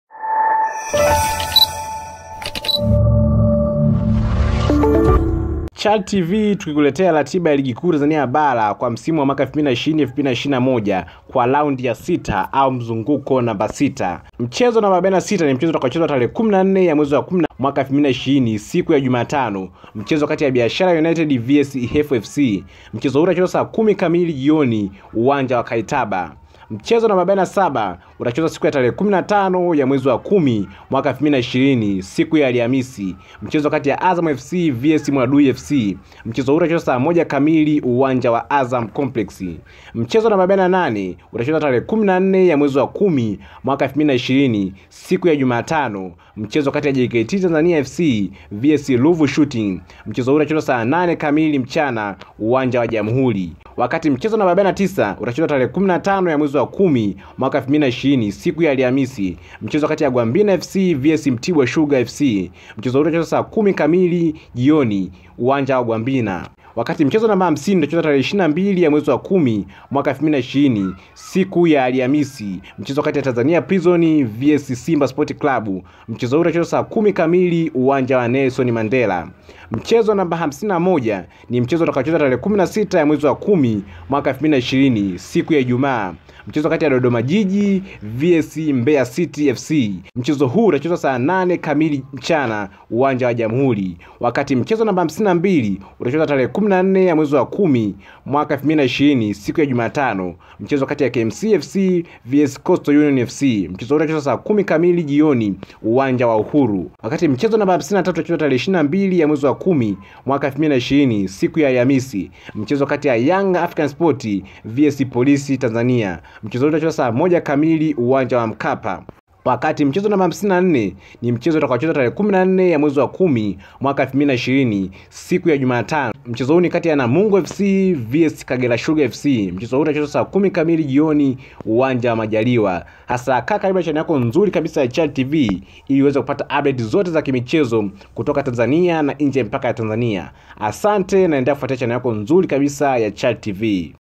RATIBA MPYA YA LIGI KUU TANZANIA BARA(2020/2021) RAUNDI YA 6/ YANGA, SIMBA MECHI ZAO KUSOGEZWA MBELE
Ratiba ya ligi kuu Tanzania bara raundi ya 6